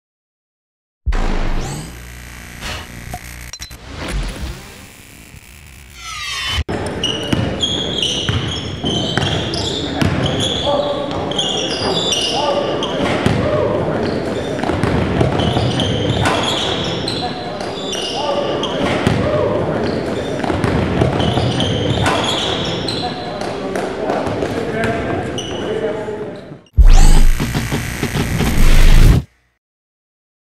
YayAreasFinest shows love for CityLeagueHoops & Saves A Dunk
My guy Travis from YayAreasFinest shows how clutch he is when he ducks and saves a Kameron Williams dunk at LeBron Camp.